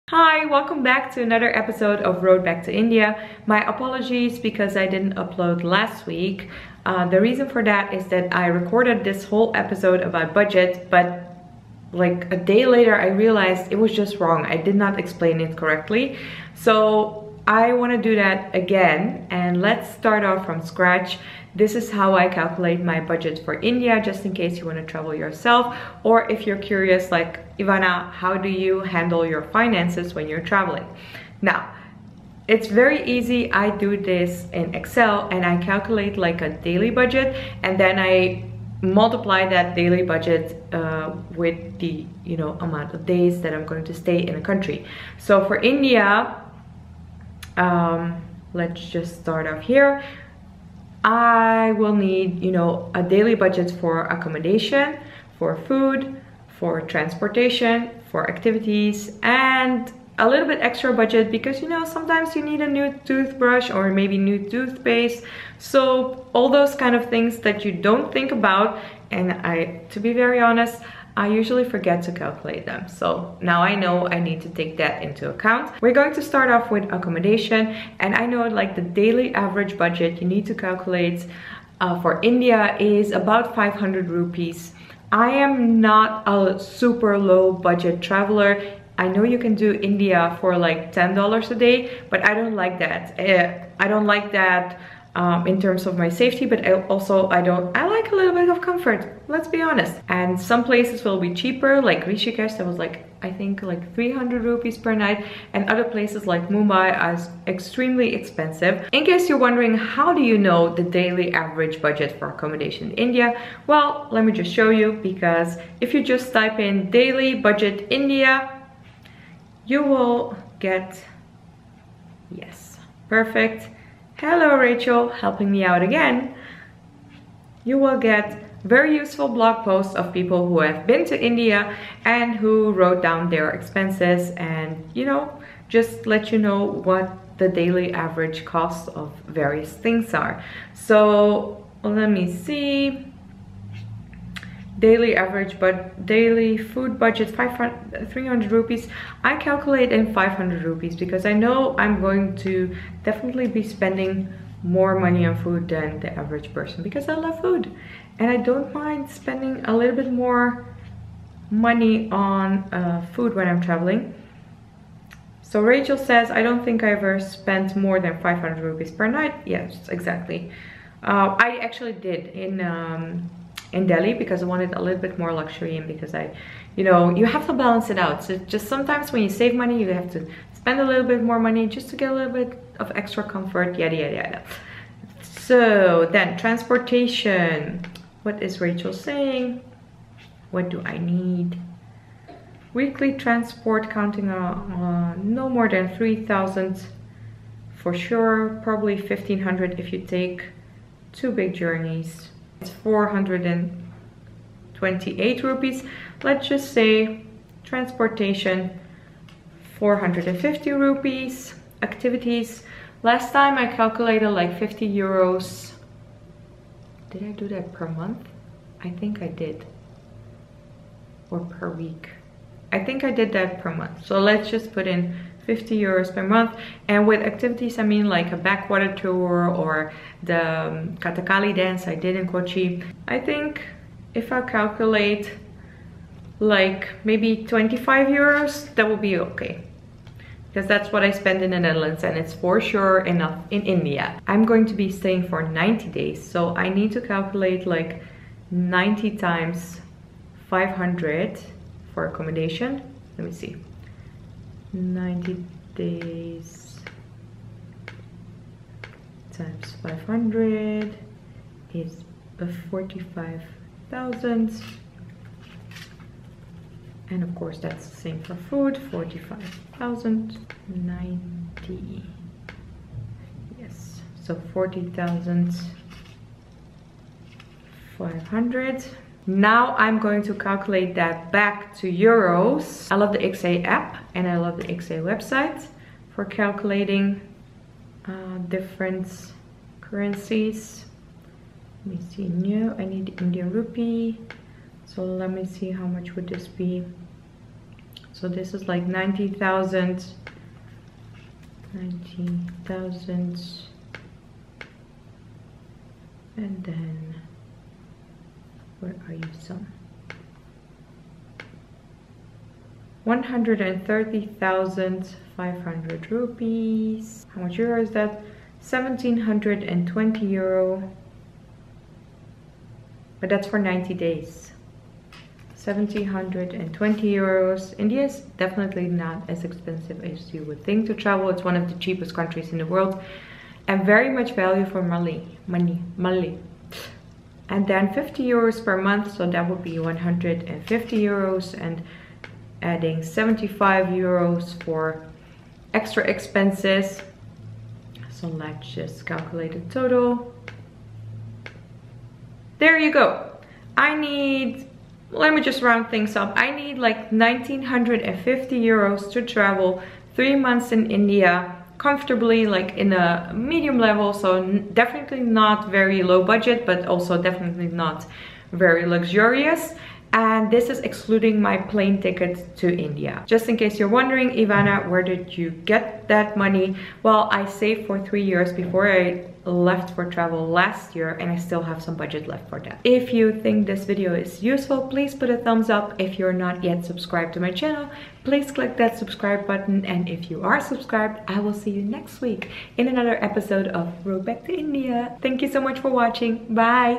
TRAVEL BUDGET | ROAD BACK TO INDIA EP 7 | ENTERPRISEME TV
I calculate my travel budget for India in this episode of road back to India. Calculating your travel budget can be tricky but this method has always worked for me.I hope this episode of road back to India is helpful for calculating your own travel budget if you want to travel.